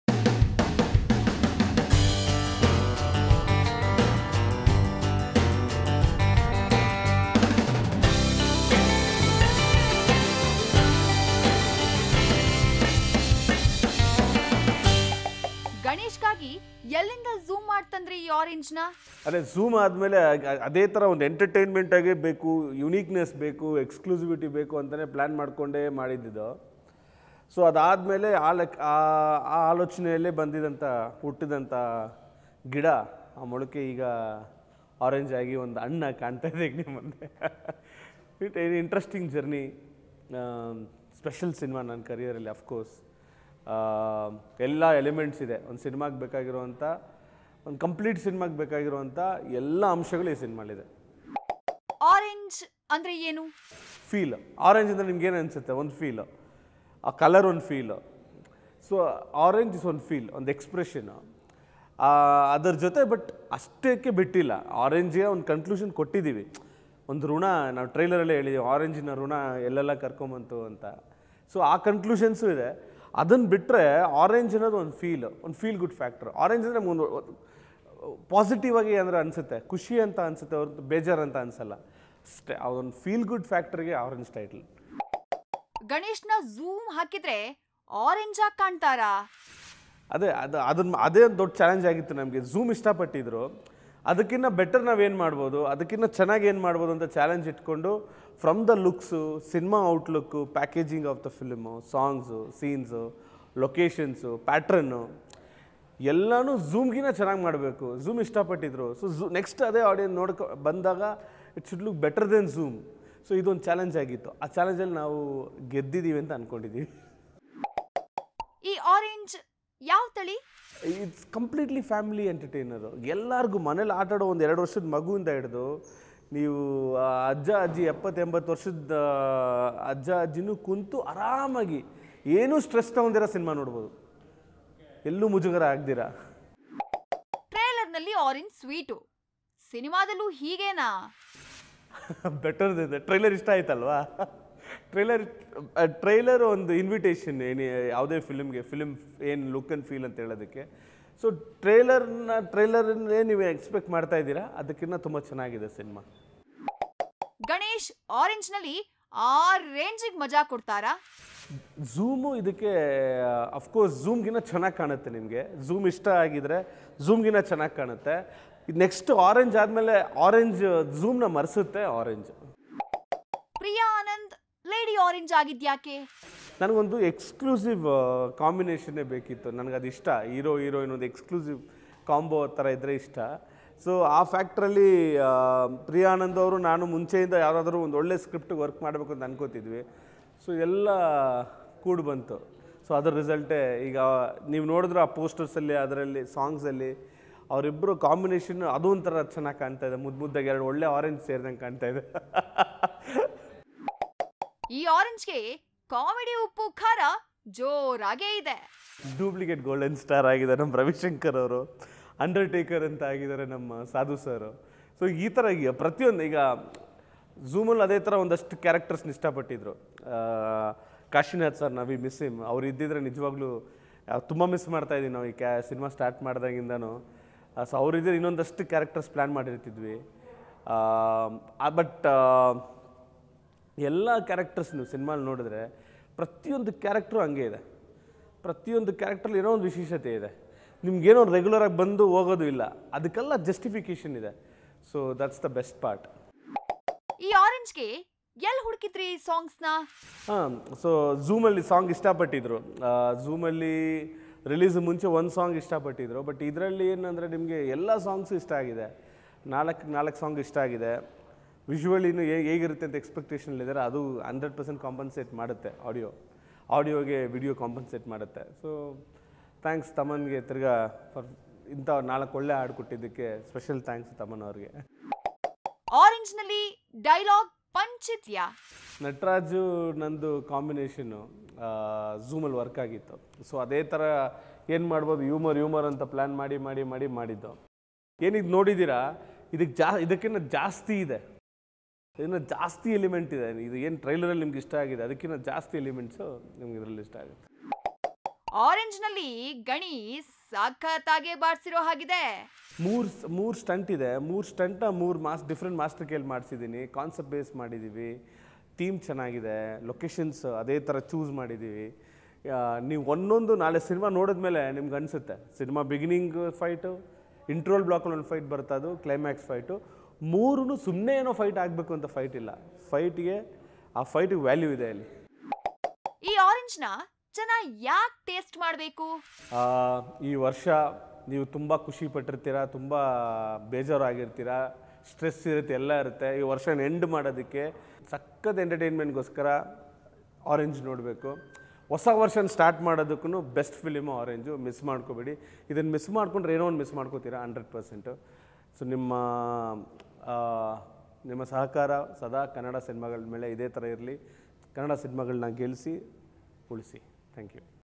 ಗಣೇಶ್​ ಆರೆಂಜ್​ನಲ್ಲಿ, ಆ ರೇಂಜಿಗೆ ಮಜಾ ಕೊಡ್ತಾರಾ?? | Orange | Prashanth Raj | Golden Star Ganesh
ಗೋಲ್ಡನ್​ ಸ್ಟಾರ್​ ಗಣೇಶ್​ರವರ ಆರೆಂಜ್ ಚಿತ್ರದ​ ಬಗ್ಗೆ ನಿರ್ದೇಶಕ ಪ್ರಶಾಂತ್​ ರಾಜ್​ ಮಾತು.

Director Prashanth Raj has spoken about his upcoming movie Orange starring Golden Star Ganesh.

FirstNews Kannada Karnataka's youngest and liveliest news channel